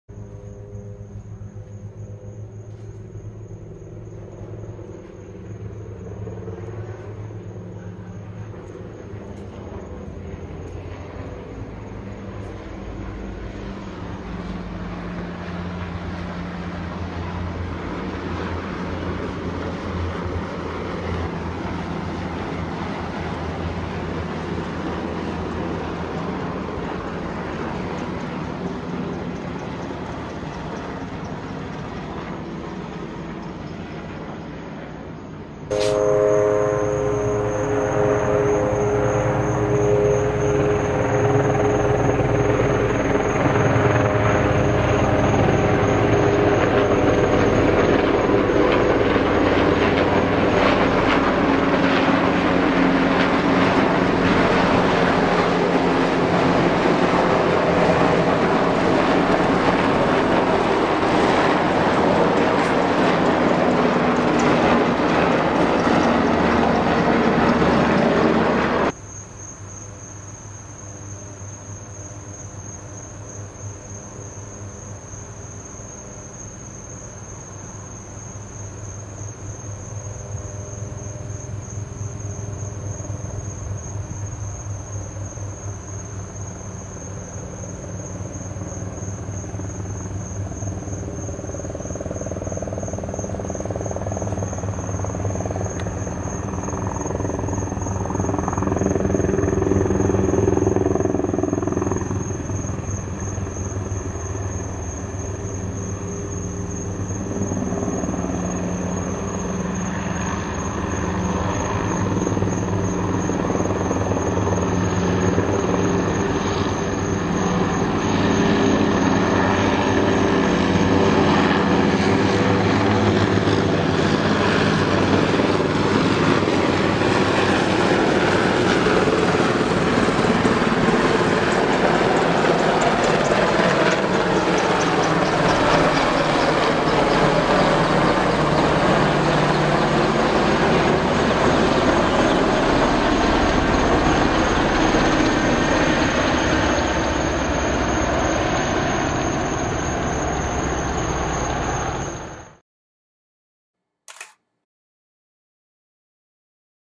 Night Time Military Training in Coral Springs, Florida (DAY 2)
March 24, 2015 around 8:20pm I heard the helicopters flying over my house again, and I found it odd, so I grabbed my phone and waited for them to pass over again so I could catch them on video. As you can see in the first iPhone video, it was extremely dark outside, so I decided to film it with my Canon 5D MKIII, but had to crank the ISO up as far as it could go, 12800. This went on until around 10:00pm.
March 23, 2015 I was shaken by the loud rumbling of 4 Black Helicopters flying over my neighborhood for about 2 hours. I was unnerved by this, because this has never happened before in my quiet & peaceful city. I did a little searching on twitter, and was notified by Coral Springs City Commissioner, Dan Daley that this was a scheduled military training operation.

According to a vague article on Coral Springs Talk, a community news website, this will be going on for the next 2 weeks.

I have lived in this city for 26 years and have never seen anything like this before.
CORAL SPRINGS IS NOT NEAR ANY KIND OF MILITARY BASE, THIS KIND OF ACTIVITY IS NOT NORMAL FOR THIS AREA

Coral Springs, Florida